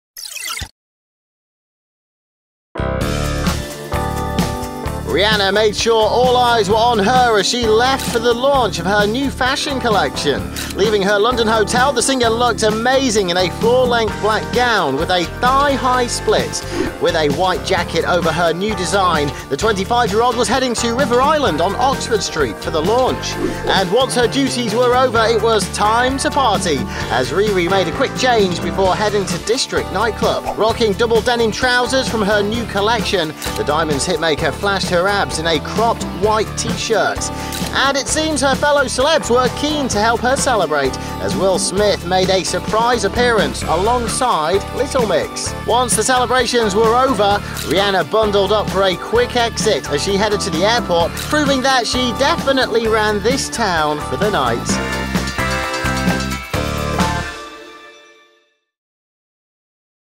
Rihanna Parties With Will Smith After River Island Launch - Splash News | Splash News TV
Rihanna made sure all eyes were on her in London as she celebrated her River Island launch.